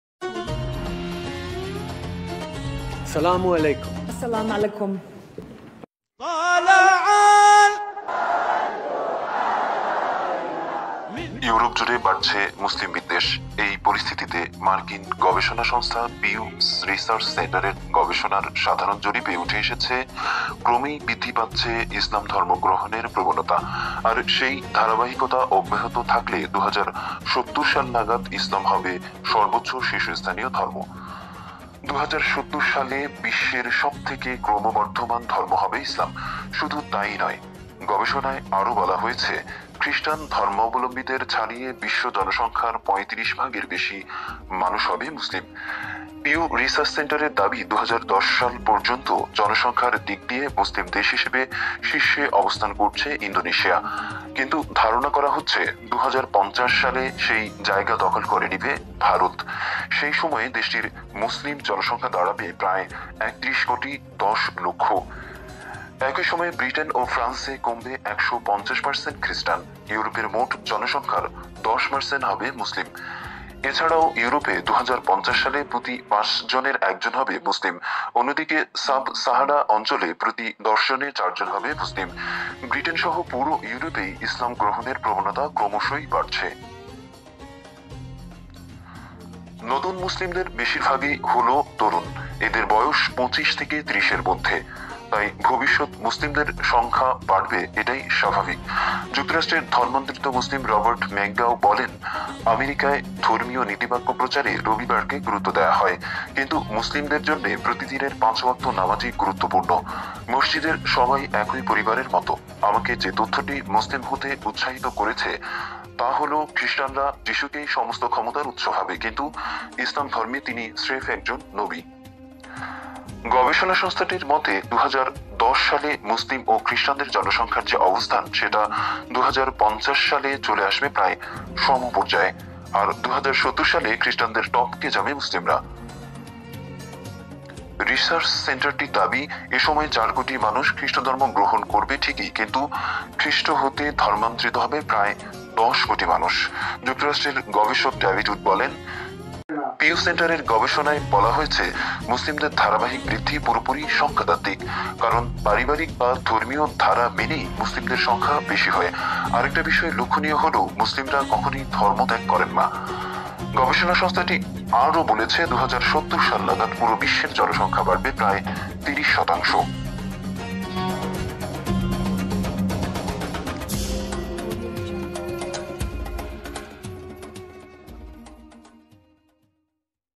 ইসলাম হবে বিশ্বের শীর্ষ ধর্ম | মার্কিন গবেষণা | The Rise of Islam | Europe
Raise of Islam in the world
Raise of Islam in Europe

Voice: Parosh Saifullah ♥
Thumbanail : Mahmud Arko ♥

√If you find any inappropriate use of your copyrighted material in this video; please contact with us.